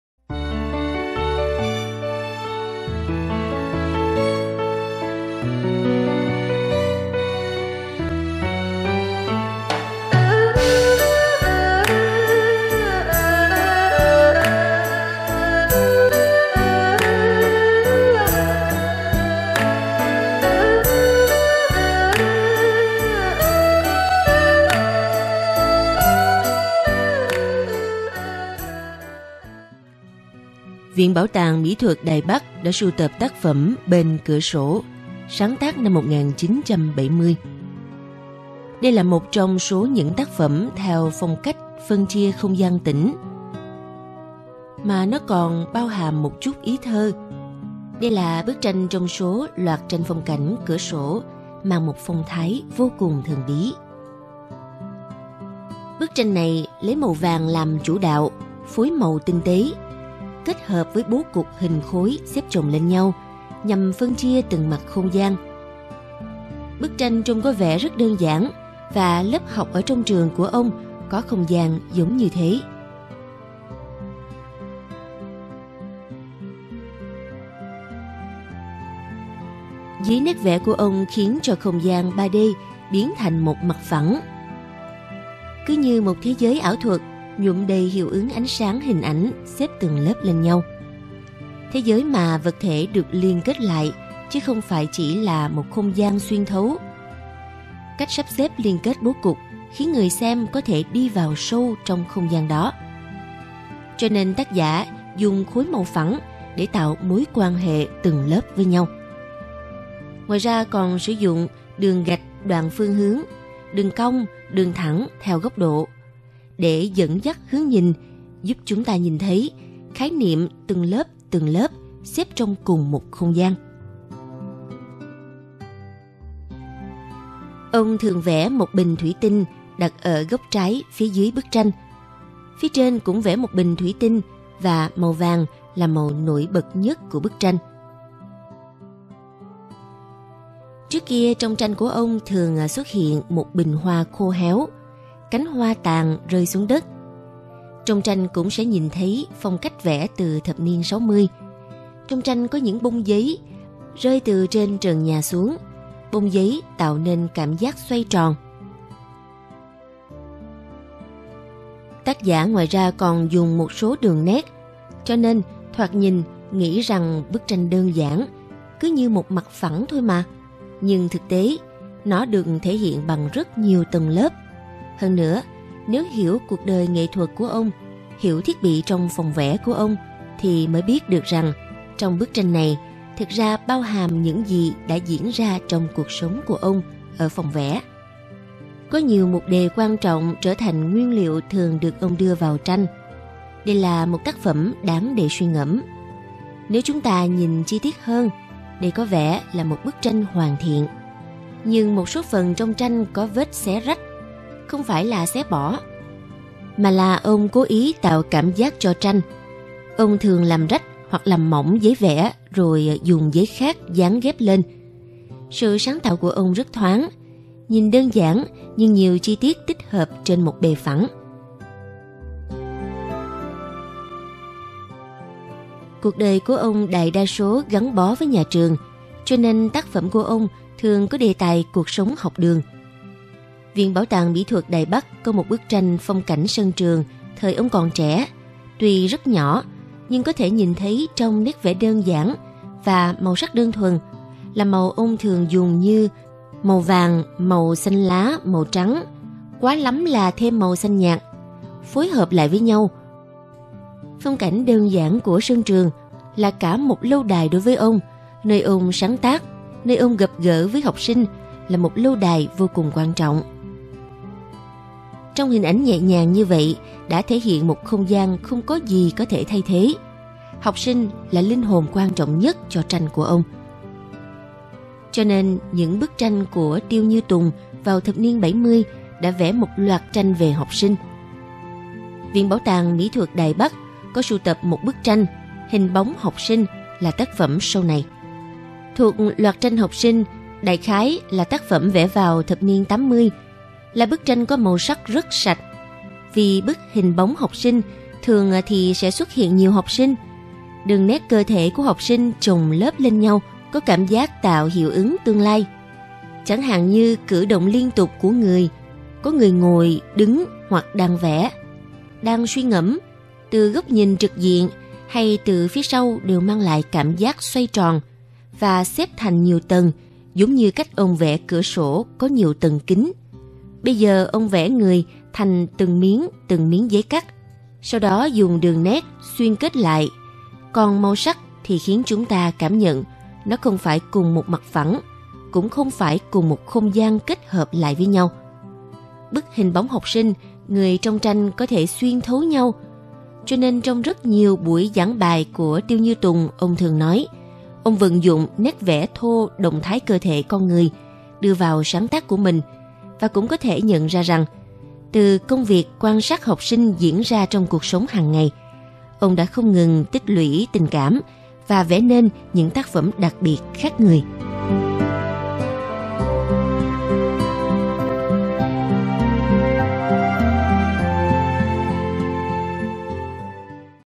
【RTI】Bảo tàng Mỹ thuật ( 23 ) họa sĩ Tiêu Như Tùng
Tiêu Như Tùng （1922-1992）
Năm 1922 Sinh ra tại khu Tây Môn Đinh Đài Bắc
Năm 1935 Tốt nghiệp Tiểu học Tầm Thường Nam Môn
Năm 1940 Tốt nghiệp Trung học Đệ Nhất Châu Lập Đài Bắc
Năm 1942 Tốt nghiệp Khoa thực tập Đại học Sư phạm Tân Trúc
Năm 1942 Làm giáo viên Trường Thiên Tuế Cao Hùng
Năm 1945 Trở về Tân Trúc tịnh dưỡng
Năm 1950 Làm giáo viên trường Tiểu học Trung Sơn Trúc Đông
Năm 1951 Làm giáo viên trường Trung học cấp 2 Khung Lâm Trúc Đông
Năm 1958 Làm giáo viên trường Trung học cấp 2 Trúc Đông
Năm 1961 Làm giáo viên trường Trung học cấp 3 Trúc Đông
Năm 1988 Về hưu
Năm 1992 Từ trần